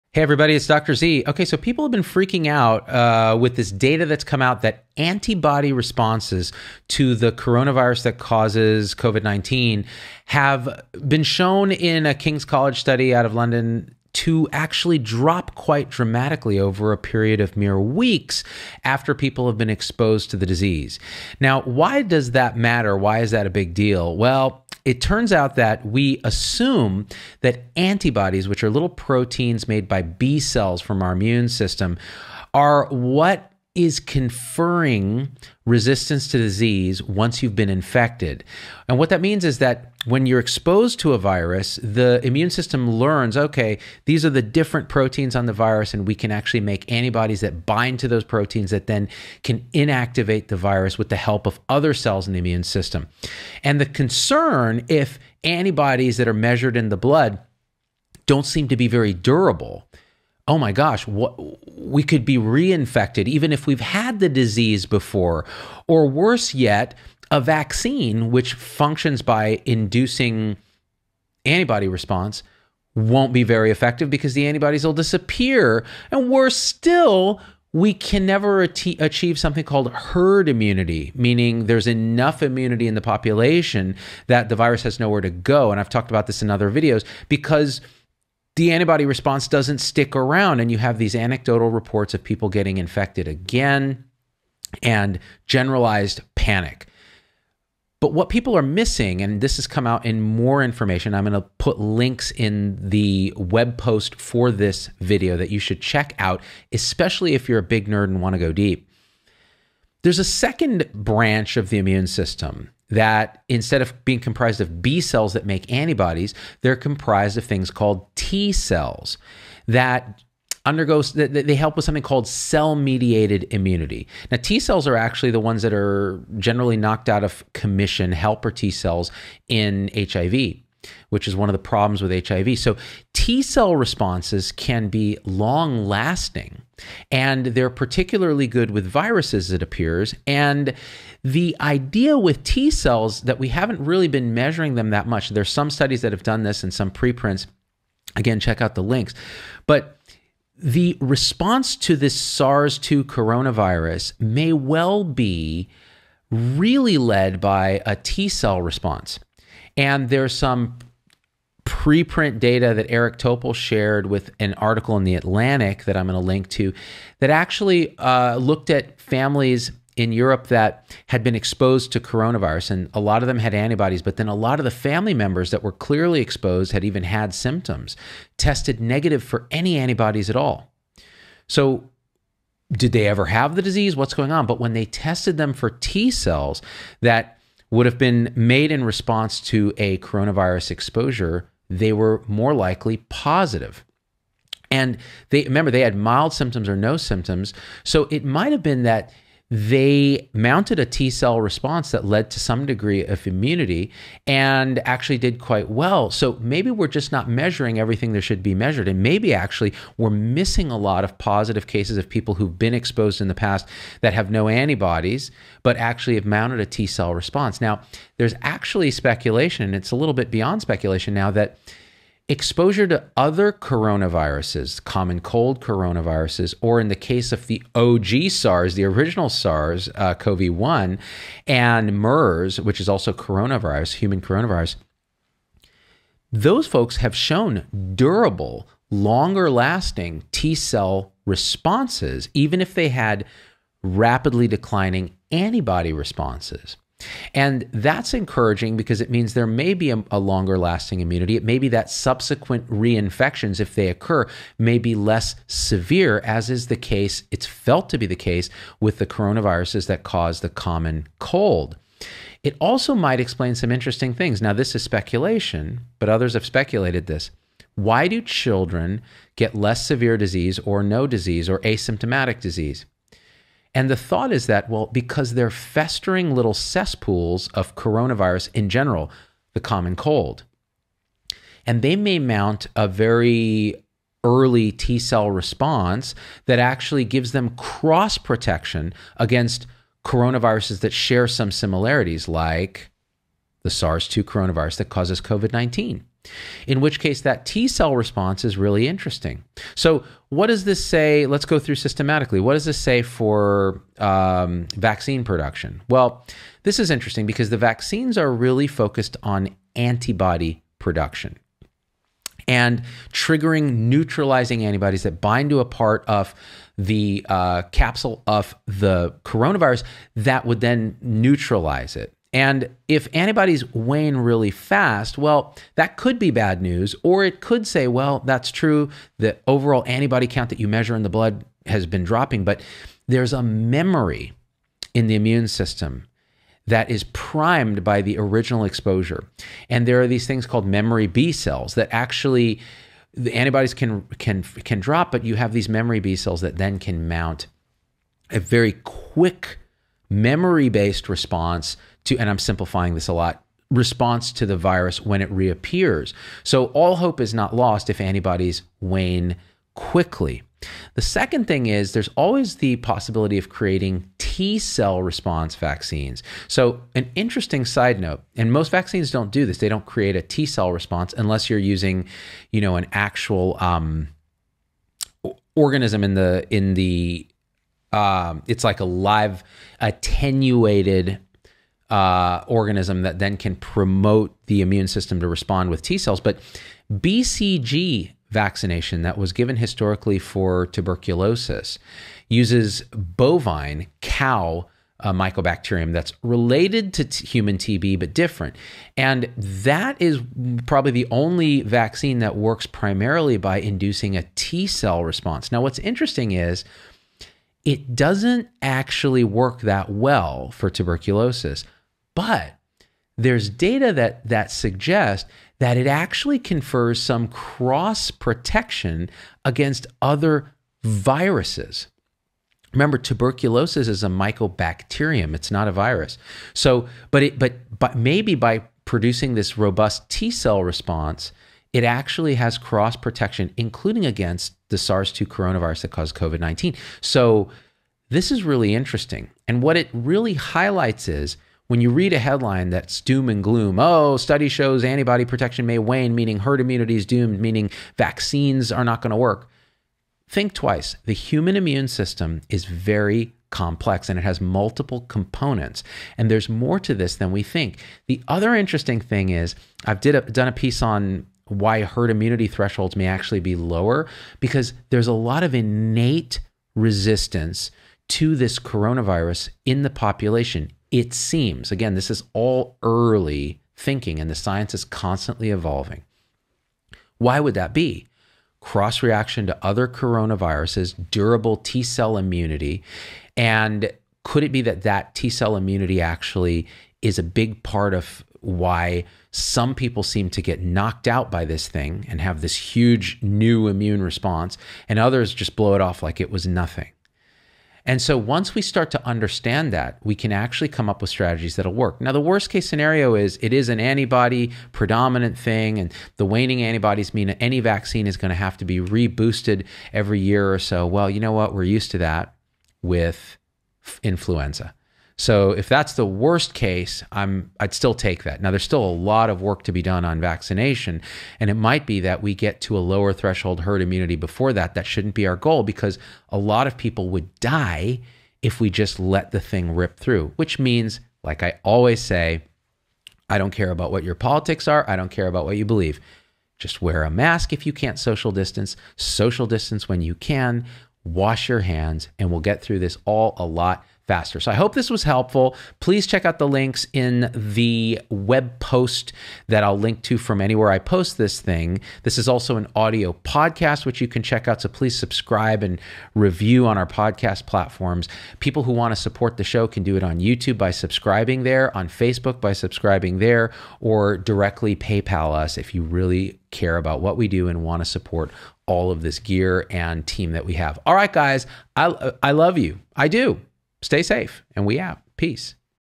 COVID Immunity Doesn't Last...Except That It Does | A Doctor Explains
There's been a lot of panic with stories of people getting COVID-19 twice and rapidly plummeting antibody levels. But there may be a real bright spot here.

Your support makes what we do possible! Join the SuperPac and get exclusive content, live discussions, and other crazy perks: